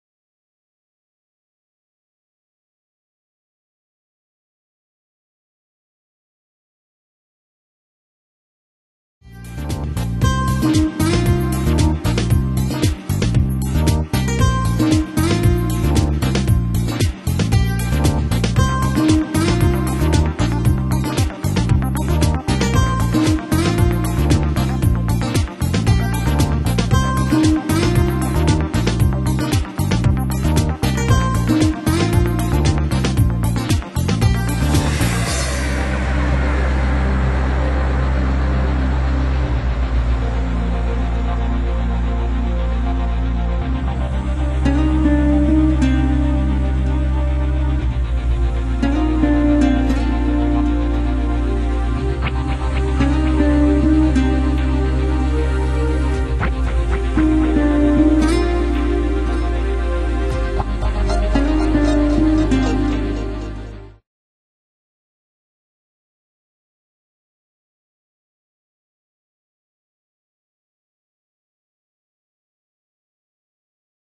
Giroauto Travel Present: Salerno City - I Giardini della Minerva di Salerno in Italy
Video realizzato per la Giroauto Travel Tour Operator di Salerno. Giroauto Travel specialisti per la Campania ed il Sud Italia. Il video di Salerno ed in particolare dei Giardini della Minerva di Salerno luogo storico della scuola medica salernitana. Per maggiori informazioni su salerno e per prenotazioni contattaci